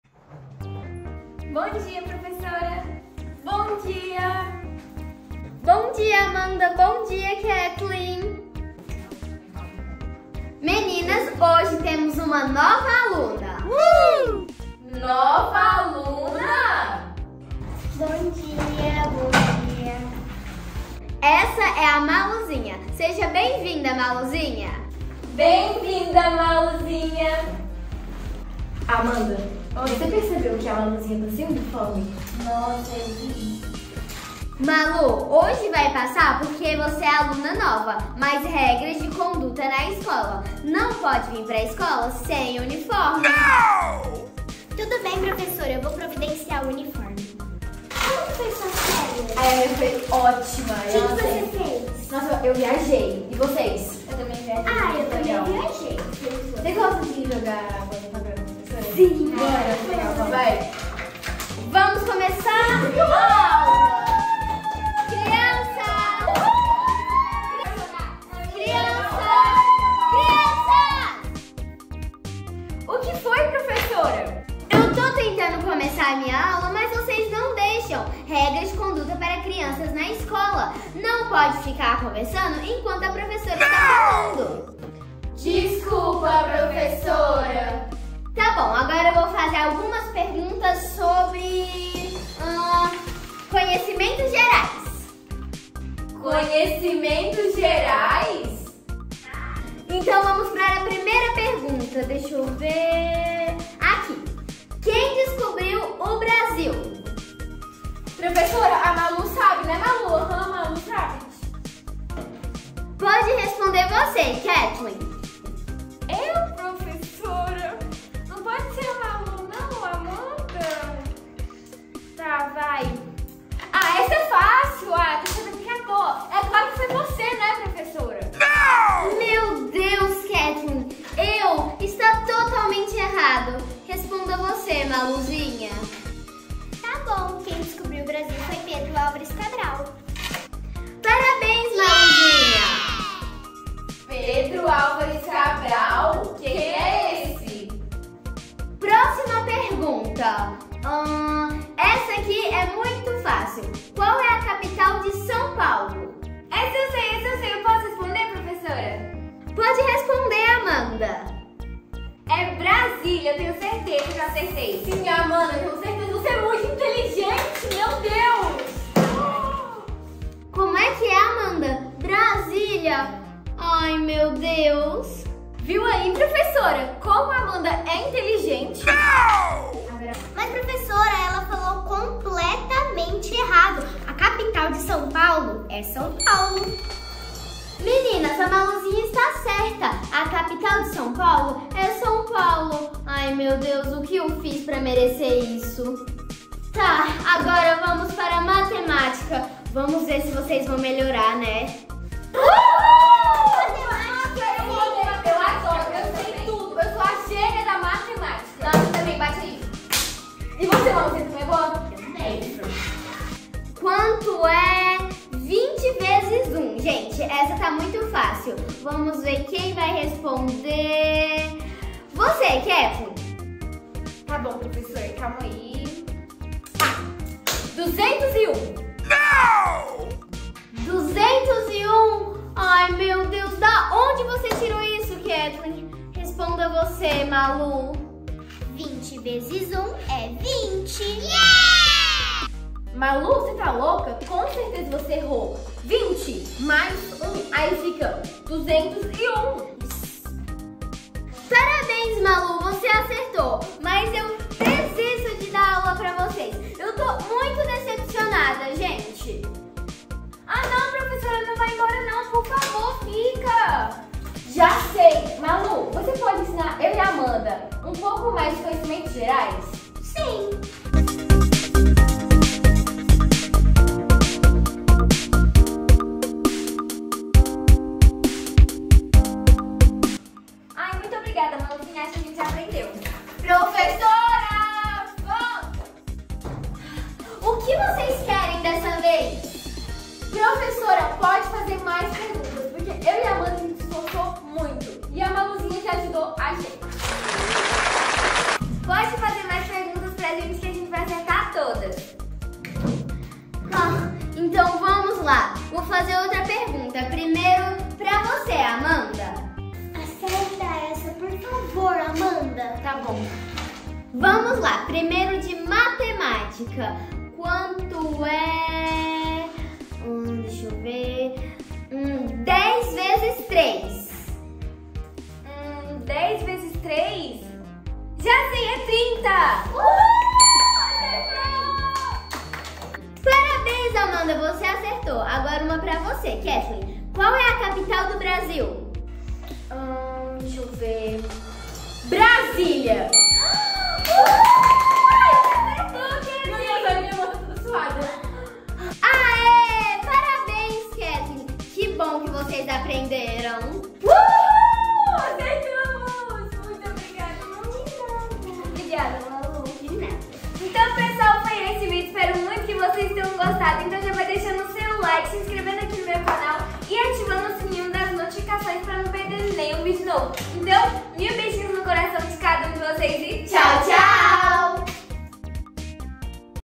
Amanda e Seus amigos aprendem regras de conduta no primeiro dia de aula - Jessica Sousa e Maluzinha
Convidei minhas amigas para gravar essa historinha de AMIGOSNAESCOLA elas pegaram o ônibus escolar e gravamos essa historinha muito divertida! Espero que gostem!

Barbie e patricinhas ensinam as regras de segurança e conduta no ônibus escolar.

Amanda e seus amigos na escola

Essa é uma História Divertida para crianças na escola.

Este vídeo não contém promoção paga todos os itens mostrados foram comprados com recursos próprios.